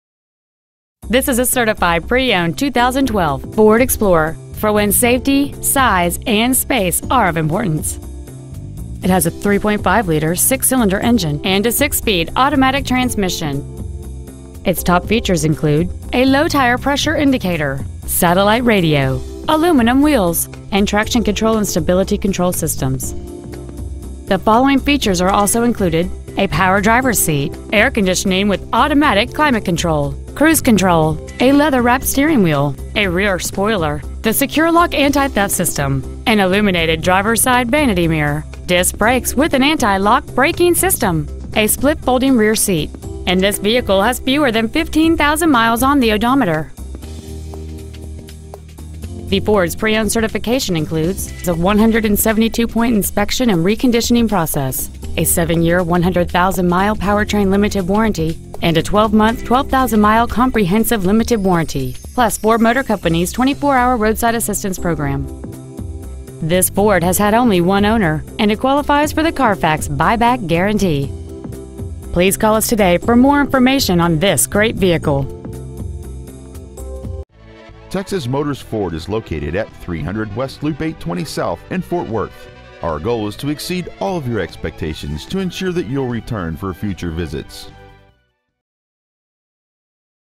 Certified 2012 Ford Explorer Irving TX
Year : 2012
 Make : Ford
 Model : Explorer
 Engine : Gas V6 3.5L/213
 Trans . : Automatic
 Exterior : Ginger Ale Metallic
 Miles : 14,449
 Interior : Charcoal Black

 300 West Loop 820 South

 2012 Ford Explorer Fort Worth TX